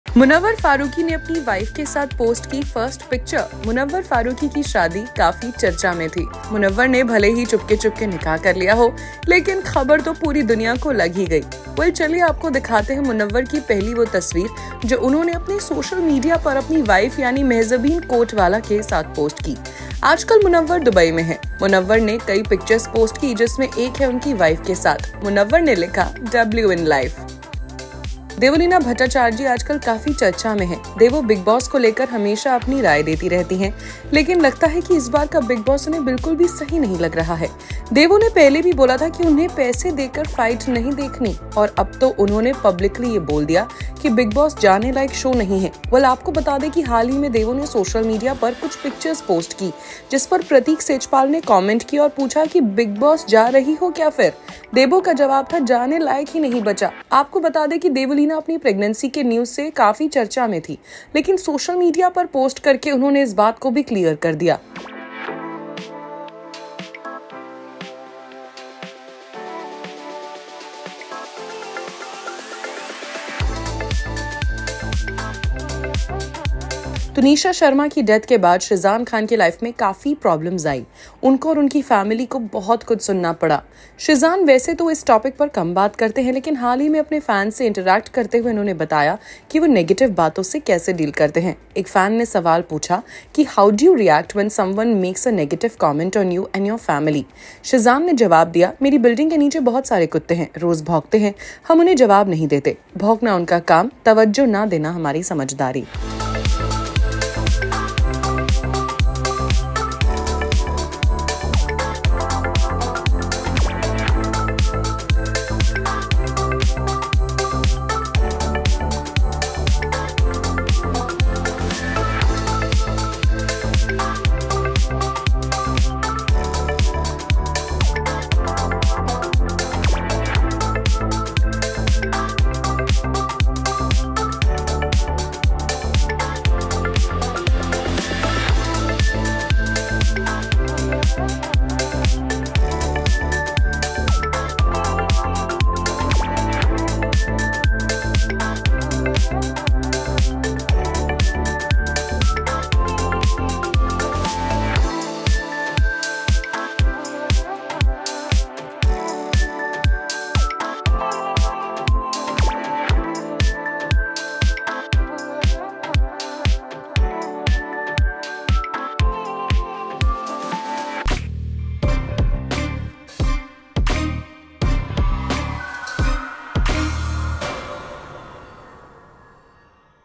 Munawar Faruqui shares FIRST pic with wife | Sheezan Khan OPENS UP on how he handles negativity
Bigg Boss 17 winner Munawar Faruqui announced his wedding to makeup artist Mehzabeen Coatwala, sharing photos on social media from their one-month anniversary celebration in Dubai. Devoleena made a surprising comment about Bigg Boss OTT 3.  Sheezan Khan discussed how he deals with negativity following the Tunisha incident.